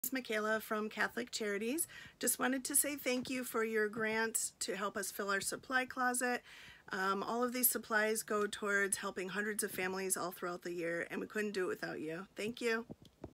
Social Service League of Ithaca 2021- 2022 Grant Celebration - Catholic Charities
The mission of the Social Service League of Ithaca is to support Tompkins County not-for-profit, human service organizations in their work to address the most significant community needs.  This year’s funding cycle was conducted in memory of Social Service League board member Audrey Cooper, who passed away earlier this year.  Typically we hold an in-person grants award ceremony in December each year but due to the pandemic we asked our recipient agencies to submit a short video on what their grant will be used for.

To learn more about the Social Services League of Ithaca visit our